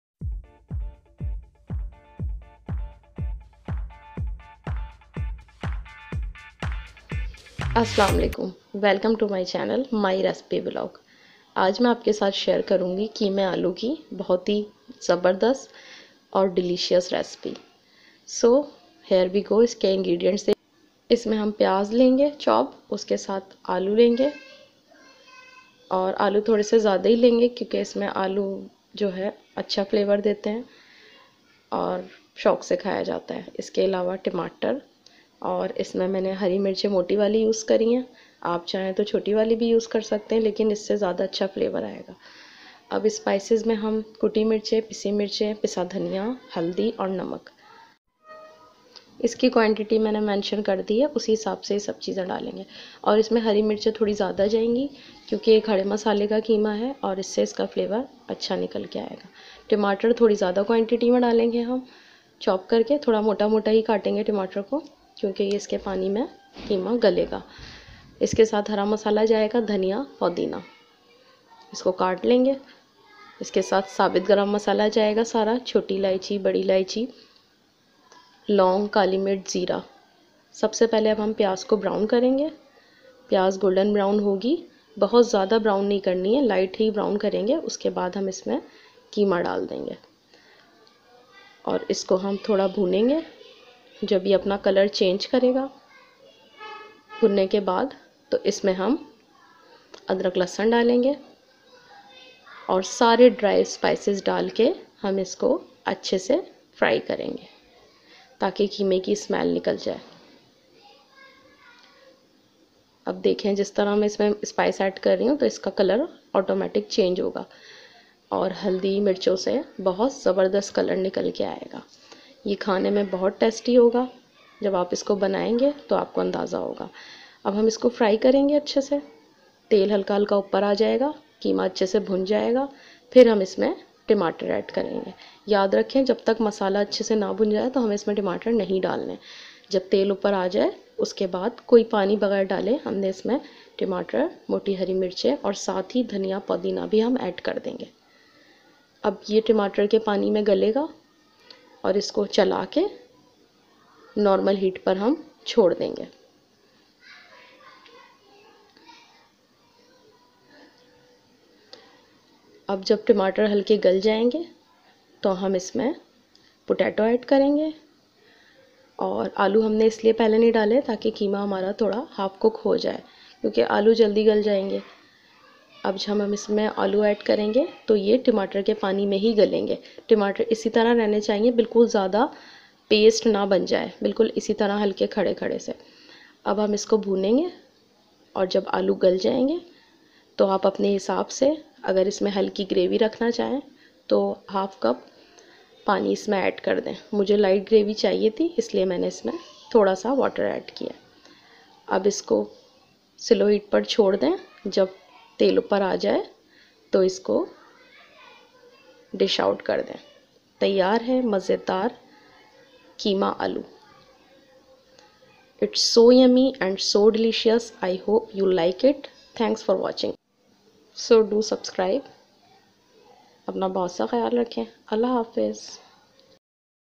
Ramadan Sehri recipe/Kharey masala ka qeema aloo recipe| gravy masala qeema aloo@mariafood84
today im sharing this delicious and mouth watering qeema aloo recipe.It full of spice and flavour and best recipe for everyone who enjoy at any occasion.Im sure you guys enjoy it mouth watering meal and like and share and subscribe my effort.Thankx to everyone who follow my style of cooking.
Kharey masala ka qeema aloo recipe| gravy masala qeema aloo@my recipes vlog.